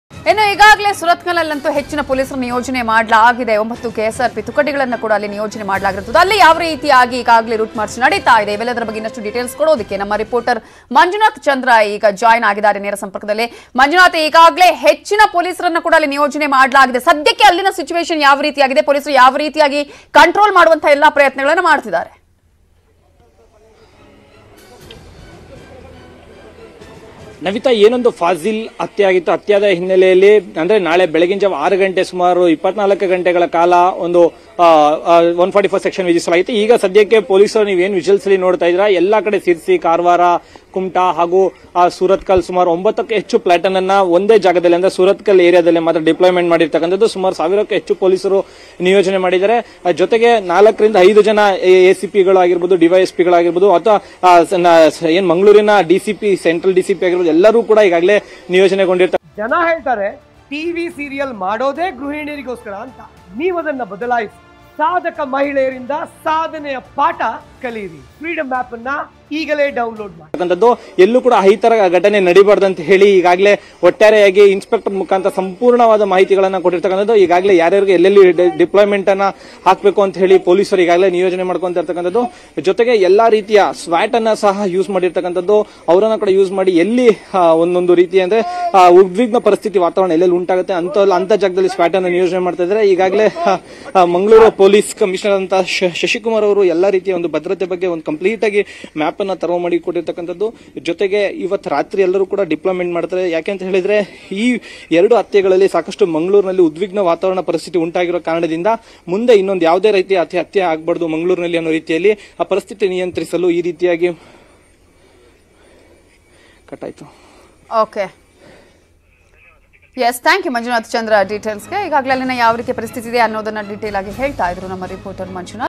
Drone ಬಳಸಿ Mangaluru ಜನರ ಮೇಲೆ ನಿಗಾ; Surathkal ಸುತ್ತಲೂ ಬಿಗಿ Police ಭದ್ರತೆ | Fazil Murder Case
ಕೃಷಿ ಮತ್ತು ಬಿಸಿನೆನ್‌ನಿಂದ ನಿಮ್ಮ ಆದಾಯ 10ಪಟ್ಟು ಹೆಚ್ಚು ಮಾಡಿಕೊಳ್ಳಲು IndianMoney ಯ ffreedom app ಡೌನ್‌ಲೋಡ ಮಾಡಿಕೊಳ್ಳಿ.